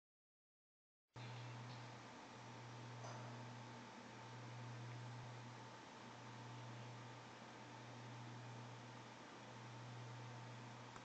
Formula 1 - Canada 2011 - Camera man falls
Camera man falls down following Rihanna.